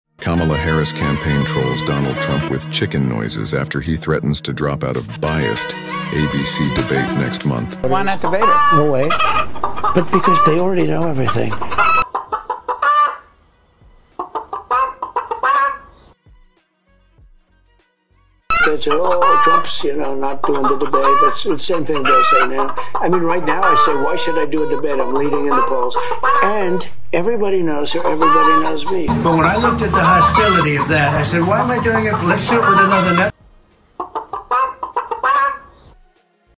Kamala Harris trolls Donald Trump with 'chicken noises'
Kamala Harris campaign trolls Donald Trump with 'chicken noises' after he threatens to drop out of 'biased' ABC debate next month
Kamala Harris has taunted Donald Trump with chicken noises after he suggested he may not take part in a debate against her with 'biased' ABC.
The debate between Vice President Harris and former President Trump is scheduled for September 10, in what will be their first-ever bout in the run-up to the Presidential election.

But on Sunday, Trump took to social media and labeled ABC 'FAKE NEWS' asking, 'Why would I do the Debate against Kamala Harris on that network?'
Also while campaigning in Northern Virginia on Monday, Trump was asked again whether he should to the debate.

He replied: 'I think ABC really should be shut out. I'd much rather do it on NBC.

'I'd much rather do it on CBS. Frankly, I think CBS is very unfair, but the best of the group. And certainly, I'd do it on Fox.'
In response, Kamala Harris' campaign team released a video of Donald Trump speaking to Fox News, in which he rails against ABC and says doesn't need to debate his rival as he has a lead in the polls, with chicken sounds added.

The Kamala HQ team posted the video on X, asking: 'You scared, Donald Trump?'

Trump tore into the network after 10pm on Sunday night in a post on Truth Social, where he gave bad reviews to the network's weekend programming before raising the threat.

'I watched ABC FAKE NEWS this morning, both lightweight reporter Jonathan Carl's (K?) ridiculous and biased interview of Tom Cotton (who was fantastic!), and their so-called Panel of Trump Haters, and I ask, why would I do the Debate against Kamala Harris on that network?' he wrote.He was referencing the network's Jonathan Karl, who questioned Trump at the White House during his presidency.

Then he went on to go after members of the network's 'This Week' panel, including Donna Brazil, a member of the DNC who parted ways with CNN after hacked emails posted on WikiLeaks in 2016 revealed her shipping debate questions to the Hillary Clinton camp.

'Will panelist Donna Brazil give the questions to the Marxist Candidate like she did for Crooked Hillary Clinton? Will Kamala's best friend, who heads up ABC, do likewise. Where is Liddle' George Slopadopolus hanging out now? Will he be involved. They've got a lot of questions to answer!!! Why did Harris turn down Fox, NBC, CBS, and even CNN? Trump wrote.Stephanopoulos apologized in July after he was caught on video speaking to a passerby about President Joe Biden. 'I don't think he can serve four more years,' the host said, days before Biden decided to get out of the race.

Trump was at it again during an appearance at a shopping center filled with Vietnamese shops and restaurants in Falls Church, Virginia.
'I think it's disgraceful, and I think ABC is a disgrace,' Trump said. 'I think having Donna Brazile sitting on that panel, she's the one that gave the answers and the questions to Hillary Clinton before a debate. I think ABC is really, should be shut out. I'd much rather do it on NBC. I'd much rather do it on CBS, frankly, I think CBS is very unfair, but the best of the group, and certainly I'd do it on Fox. I'd even do it on CNN,' Trump said.

He again complained about the ABC roundtable, saying the 'hostility is crazy.' Trump called Harris a 'dummy.'

The two camps continue to haggle over the terms of the upcoming debate, the only one left that both sides have agreed to.

Harris advisor Brian Fallon said both candidates should have their mics kept live throughout the broadcast.

'Our understanding is that Trump's handlers prefer the muted microphone because they don't think their candidate can act presidential for 90 minutes on his own,' he said in a statement. The Trump camp, meanwhile, says Harris wants a seated debate that allows for notes and opening statements.

The Harris camp disputes this. 'All three parties (Trump, Harris and ABC) have agreed to standing and no notes, and we never sought otherwise,' said Fallon.